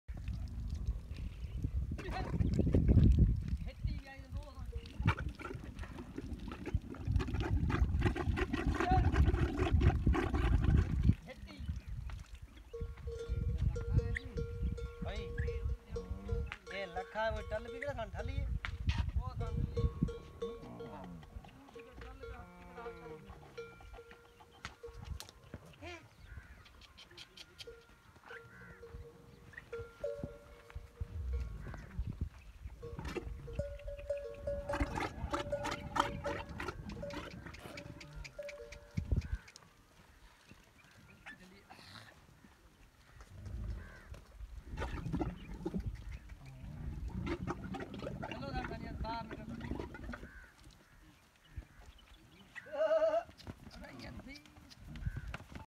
A male camel is preparing to bluetooth a female camel | جمل ذكر يستعد للبلوتوث جمل أنثى
H A male camel is preparing to bluetooth a female camel | جمل ذكر يستعد للبلوتوث جمل أنثى